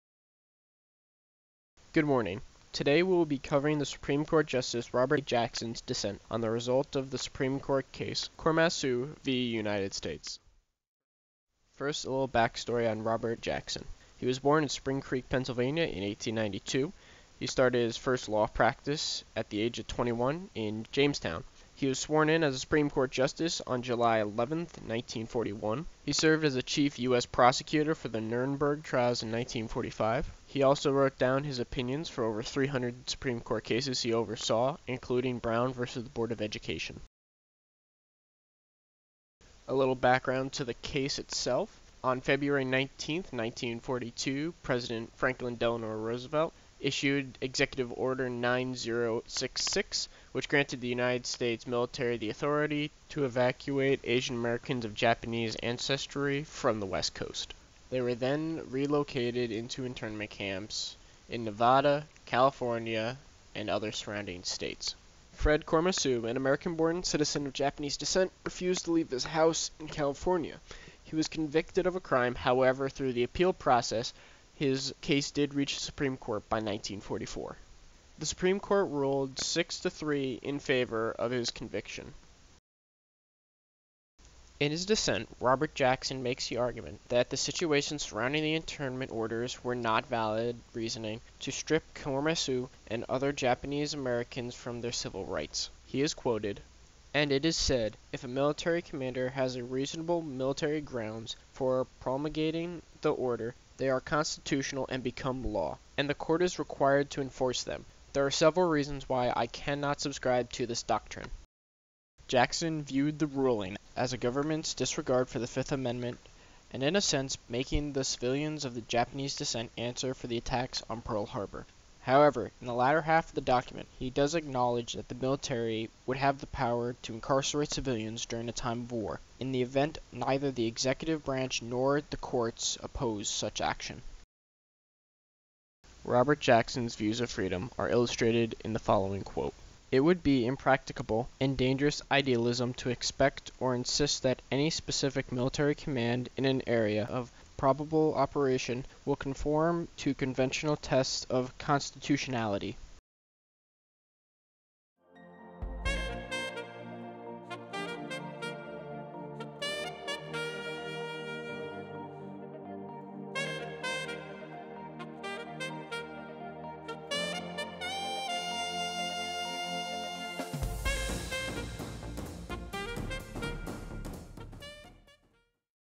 Robert Jackson's Dissent on Korematsu vs United States
A short video about Robert Jackson's dissent on the supreme count case Korematsu vs United States.